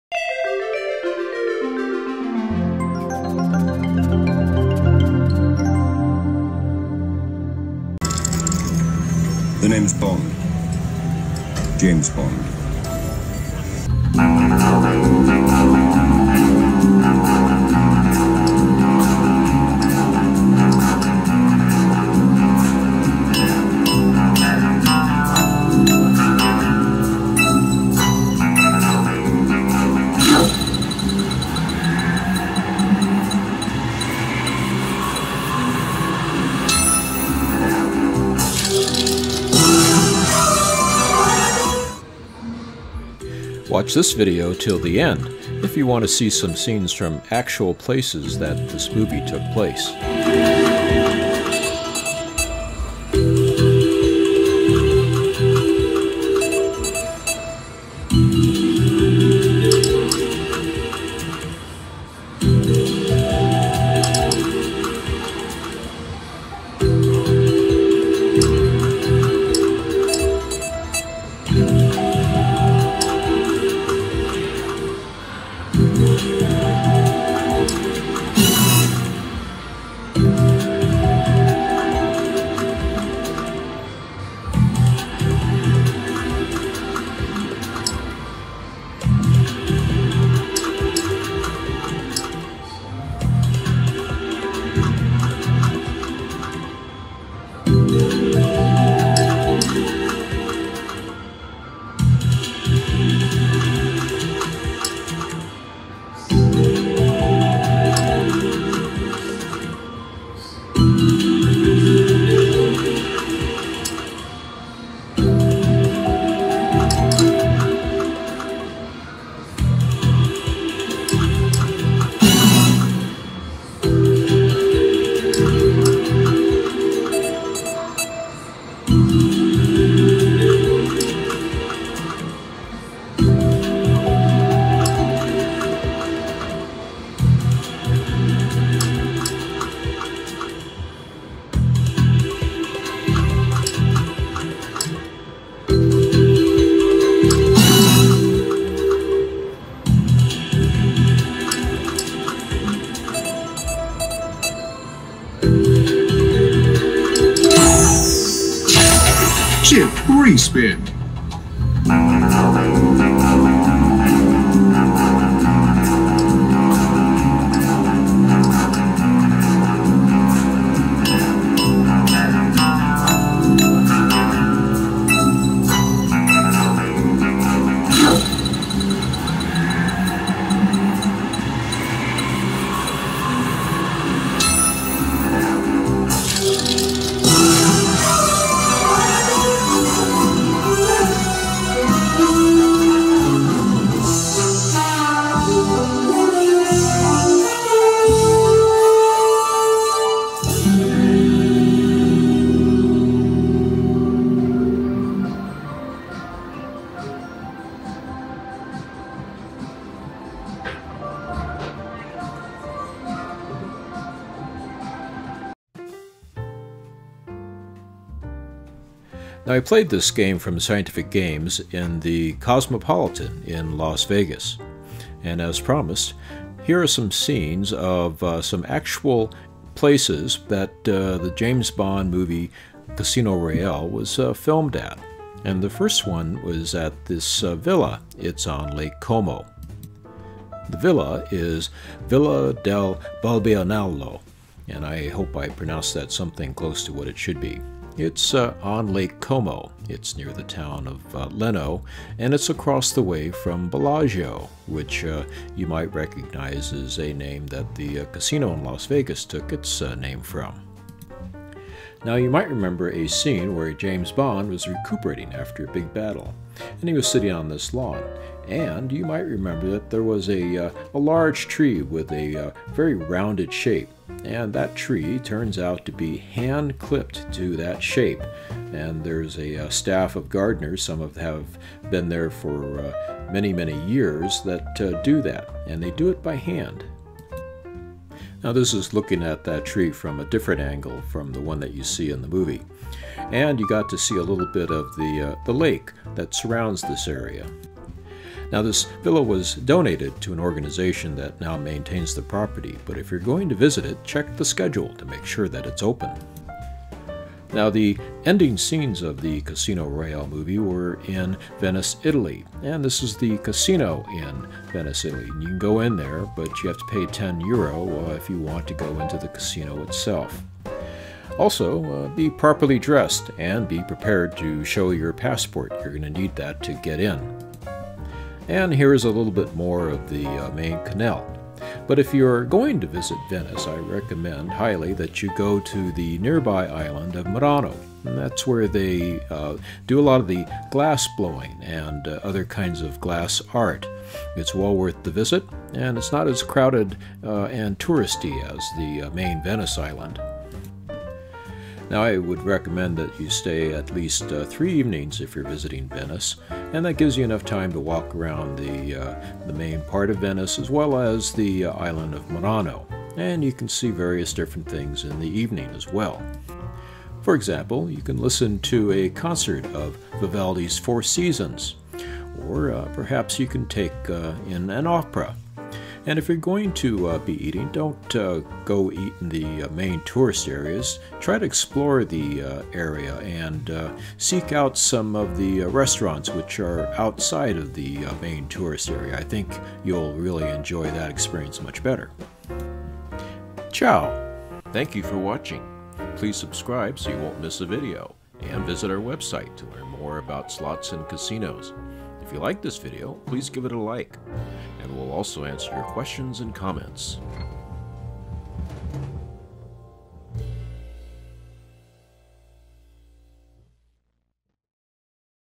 Casino Royale slot machine bonus & places the movie was filmed
Cosmopolitan casino in Las Vegas / Scientific Games
and video of Villa del Balbianello and Venice in Italy where Casino Royale was filmed.